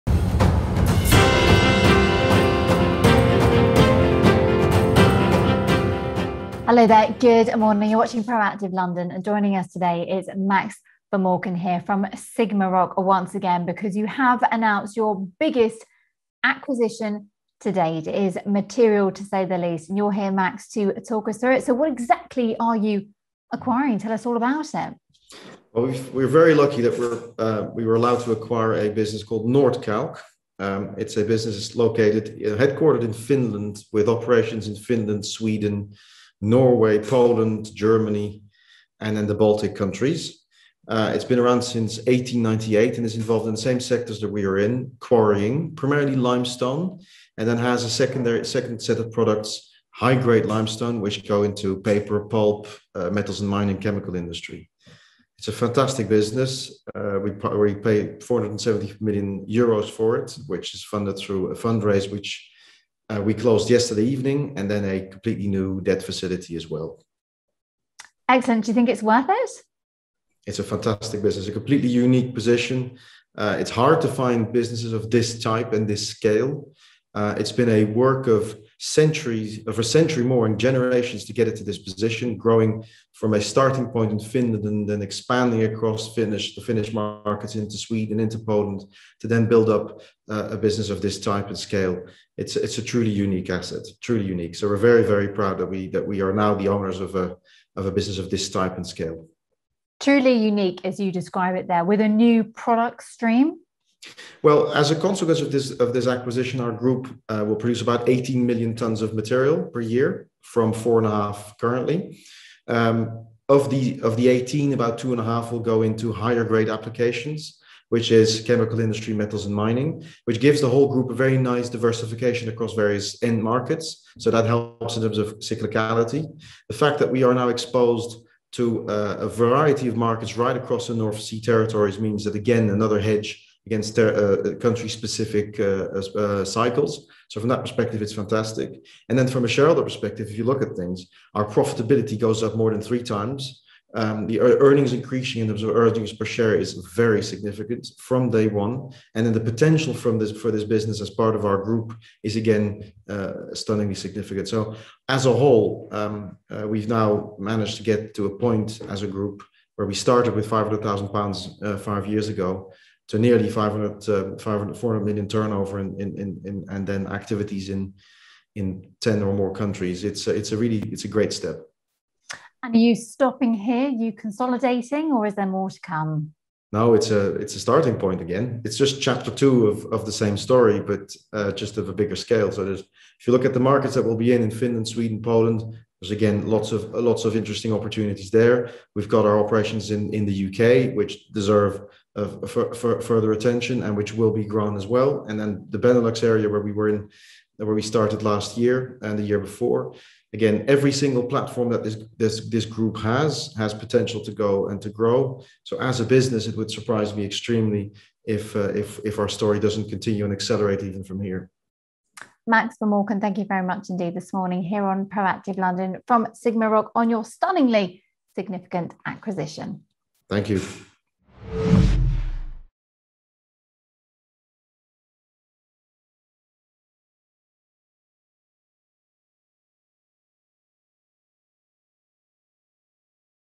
SigmaRoc acquire Finnish limestone quarries group Nordkalk 'a truly unique asset'
SigmaRoc PLC (LON:SRC) Max Vermorken, the chief executive of SigmaRoc talks to Proactive after conditionally agreeing to purchase Nordkalk, a wholly-owned subsidiary of Rettig Group.

SigmaRoc agreed to pay about £402mln for the Finnish limestone developer in what is classified by AIM as a reverse takeover.

The AIM-quoted group will issue around €50mln (roughly £43mln) of shares to Rettig with the rest of the consideration to be satisfied through cash.